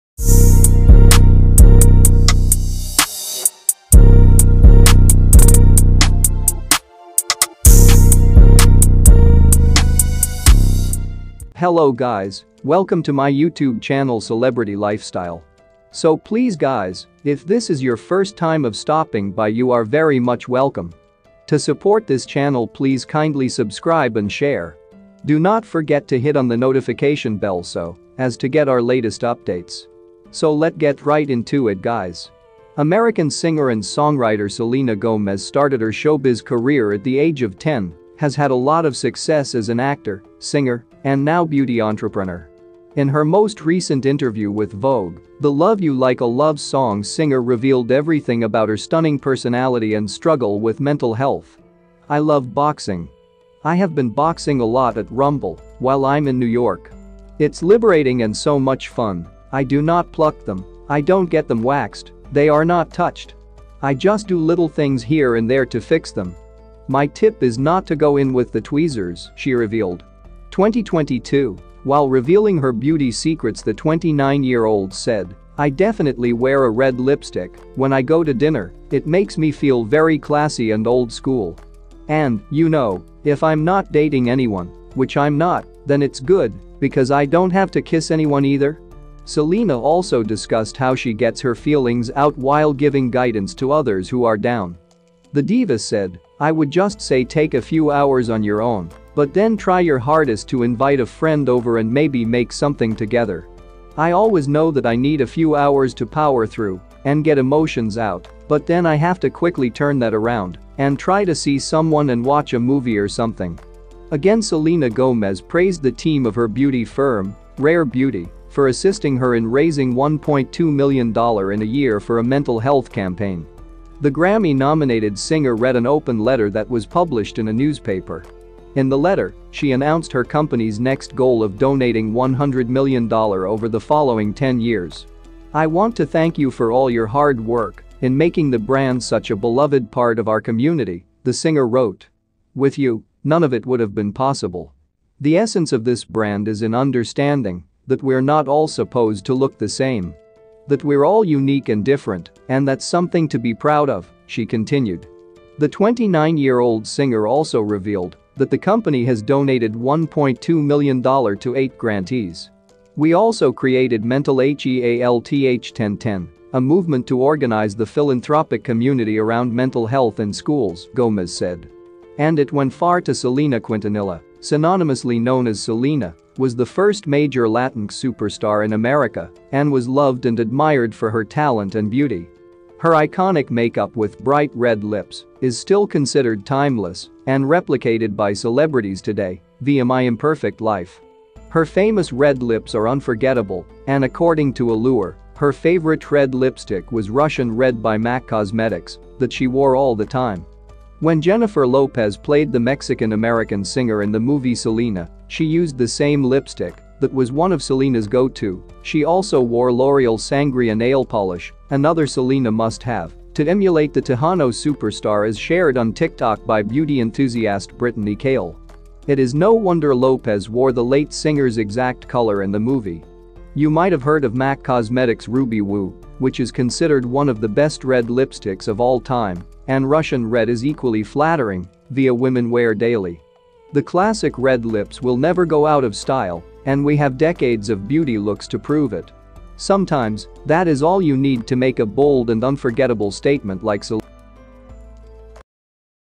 Selena beg for forgiveness on the behalf of keeping her Mental health issue as a secret
in this video we are going to expose it all to you guys just how Selena Gomez begs for forgiveness and review all the secret behind her Mental health